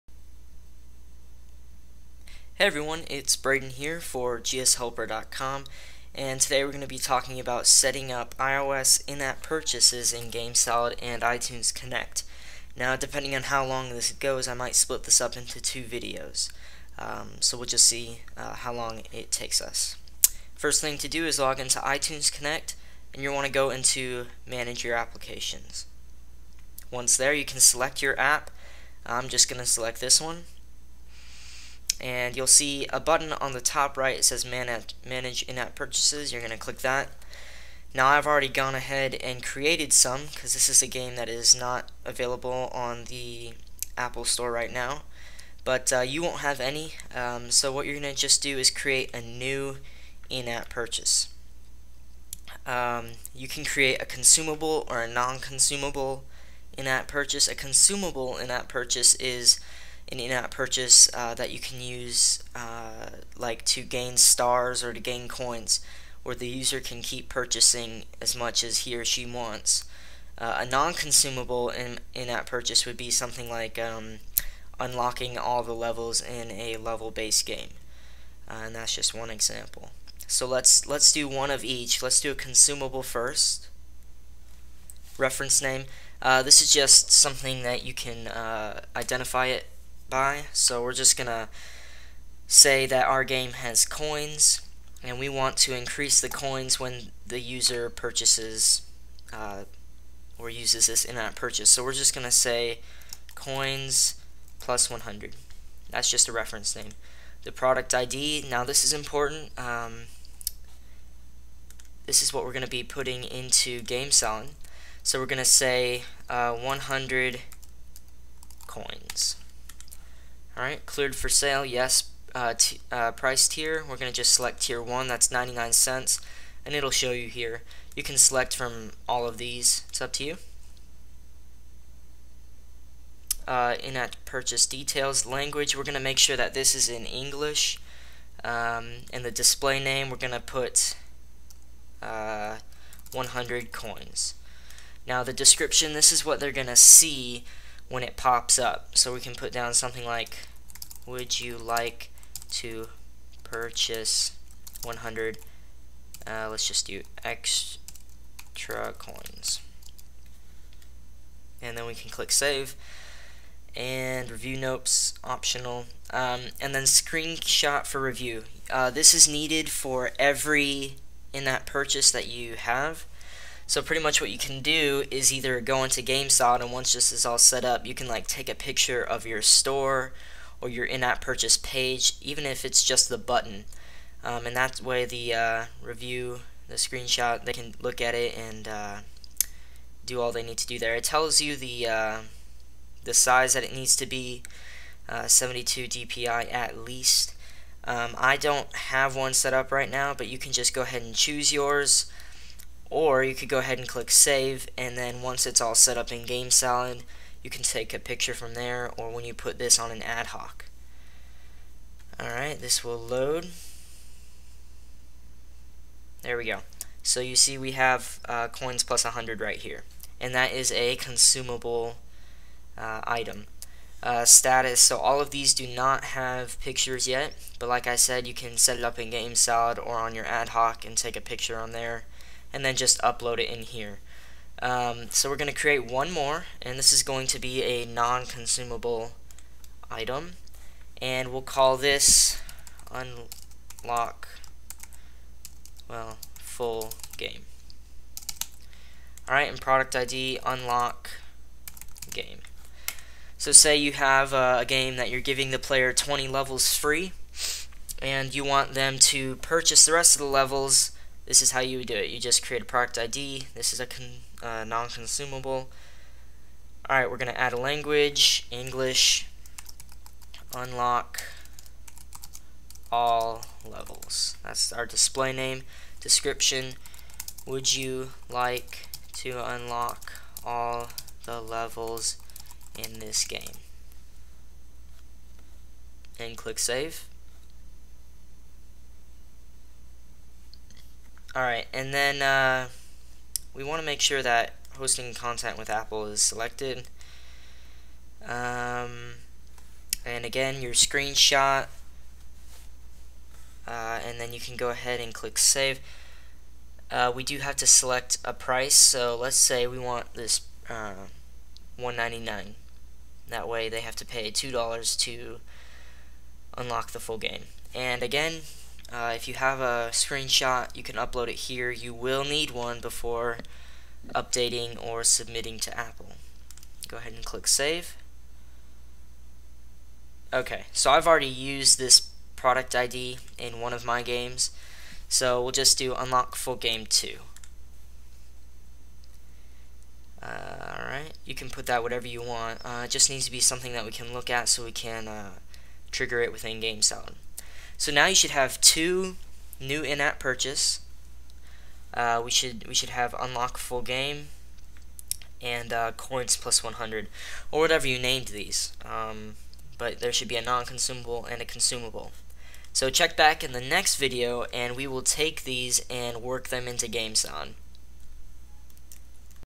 How to set up iOS In-App Purchases in GameSalad - Part 1
Hey everyone,

In this video, we show you how to set up iOS In-App Purchases in iTunes Connect. This is part 1 of 2 in the video series. Enjoy!